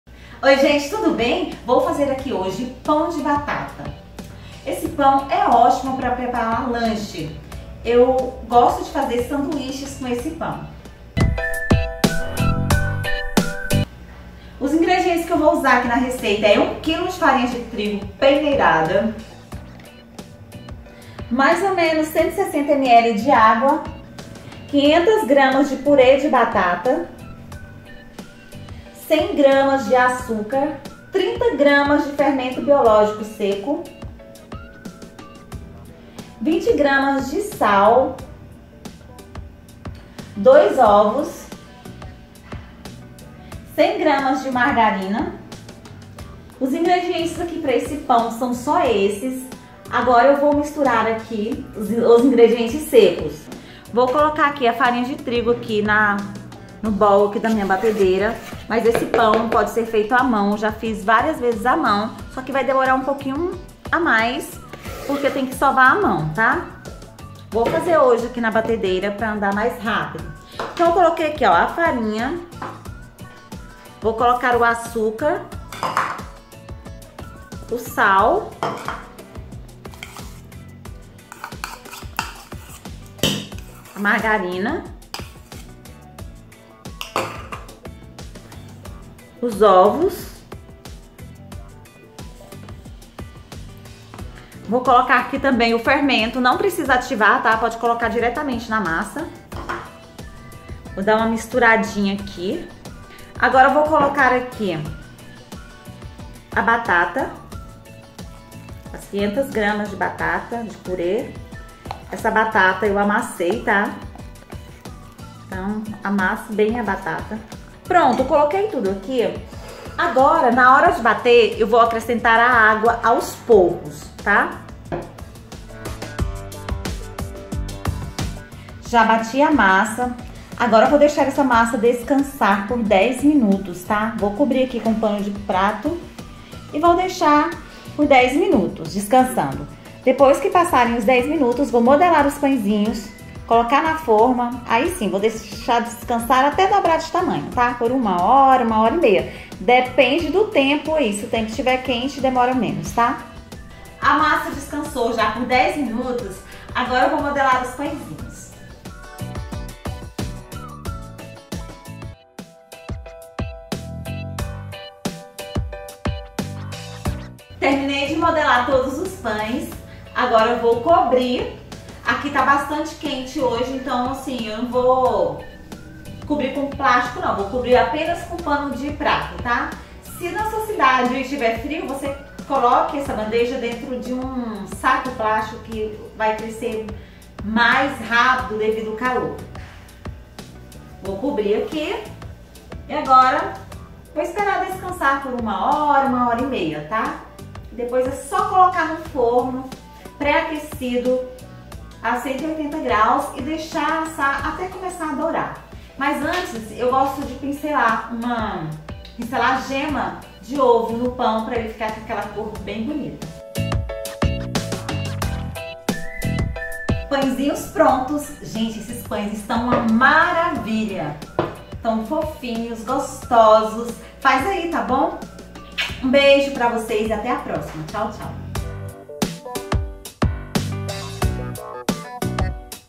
Pão de Batata Caseiro
Ingredientes:
  1kg de Farinha de Trigo
  +- 160ml de Água
  100g de Açúcar (5 colheres de sopa)
  500g de Pure de Batata  inglesa (cozida e amassada)
  20g de Sal (1 colher de sopa)
  2 Ovos
  3 Colheres de Sopa de Margarina
  30g de Fermento Biológico Seco